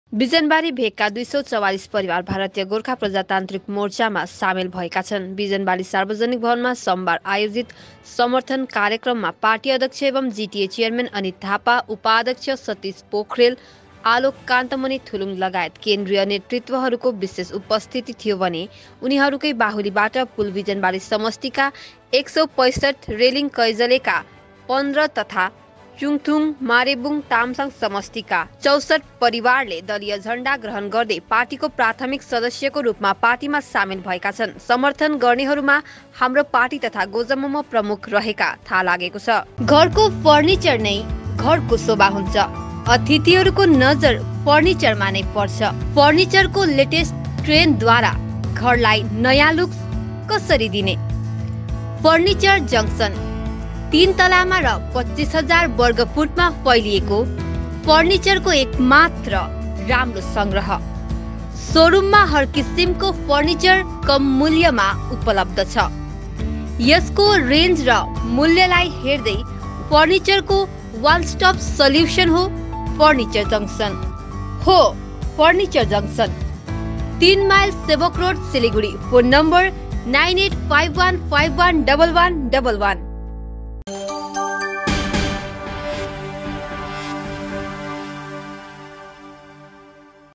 Several households in Bijanbari take up BGPM flag
विभिन्न पार्टी परित्याग गरि २४४ परिवारले समाए भारतीय गोर्खा प्रजातान्त्रिक मोर्चाको झण्डा